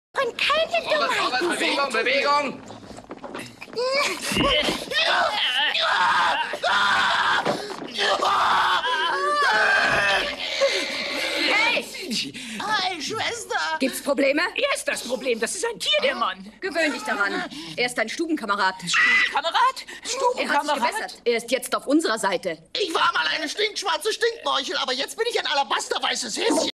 Zed and Sweetchuck in their room
Zed and Sweetchuck in their room, Sgt Callahan joining